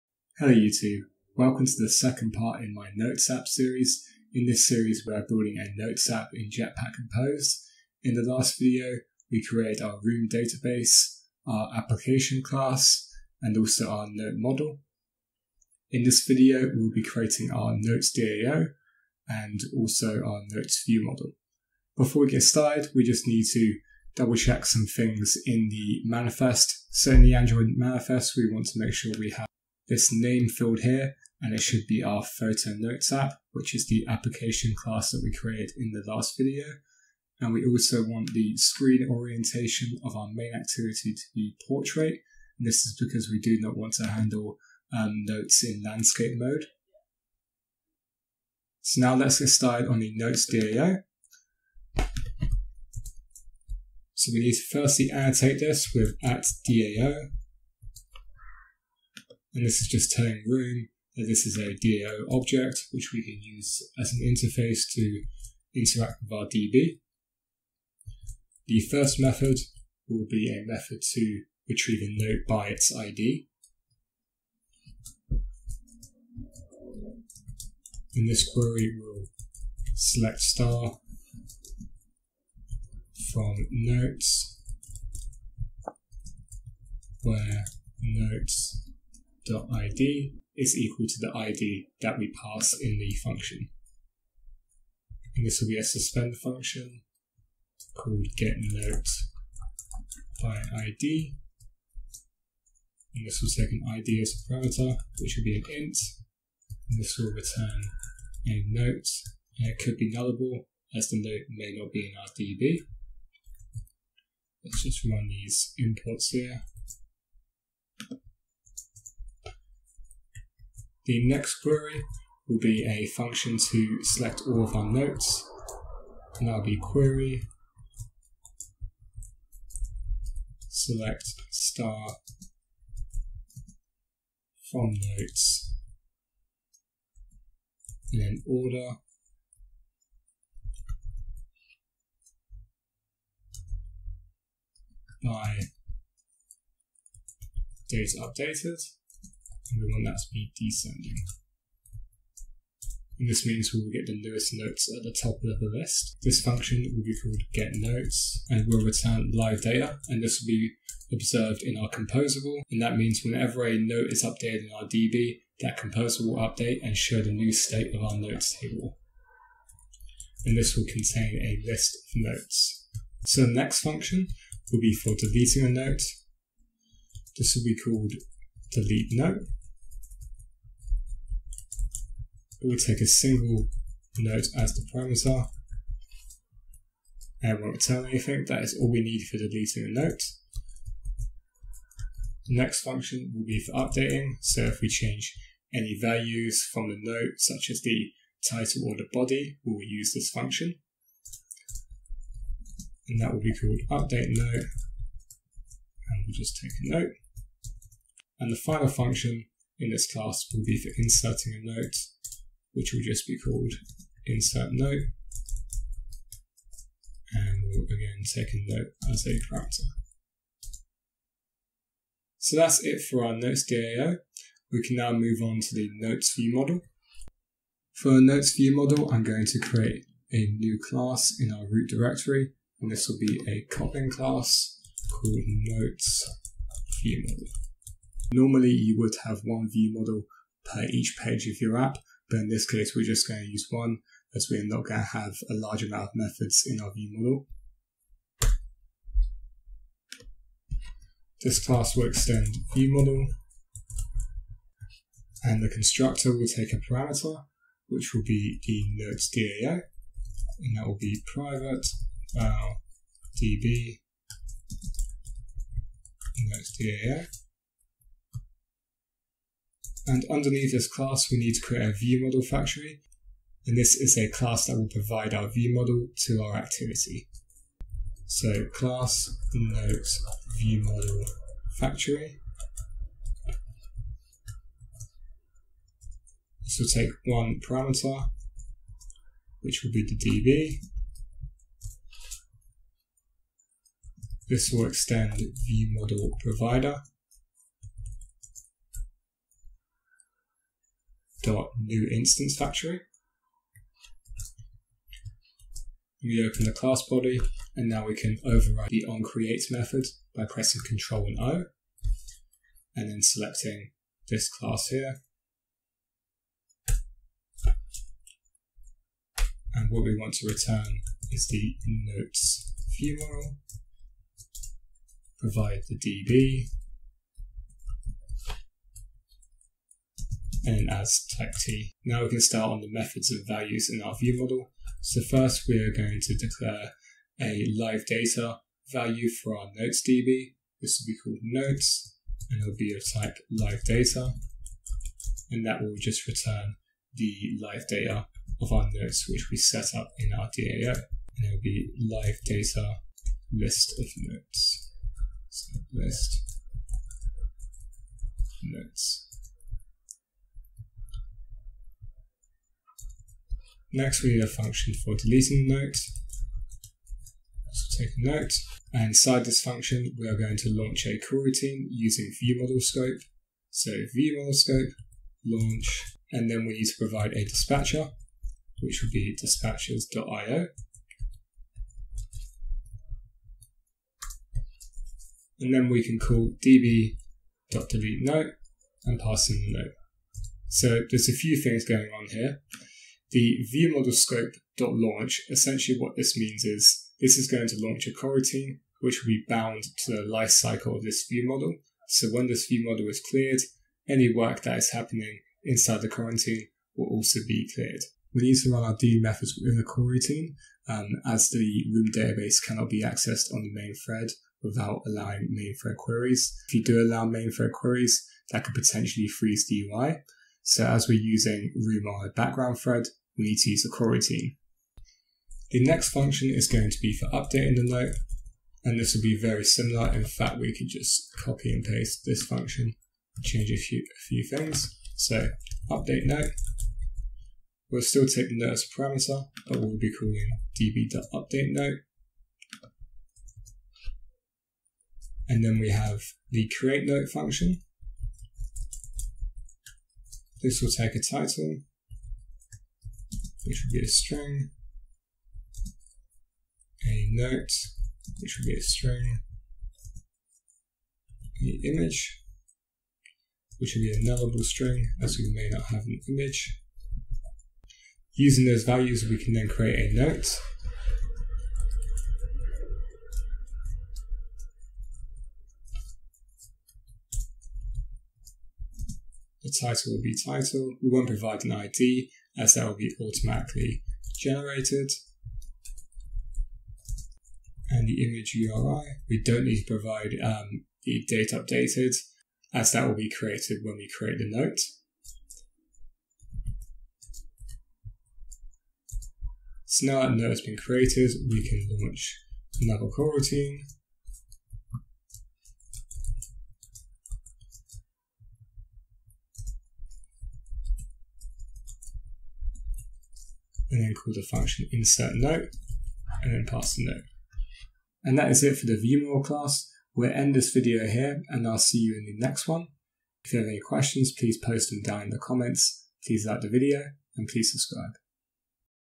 Android Jetpack Compose Notes app with Photos Part 2
In this video, I will be setting up the Notes Dao and also the Notes ViewModel.

Timestamps
(00:00) Application class
(00:51) Notes DAO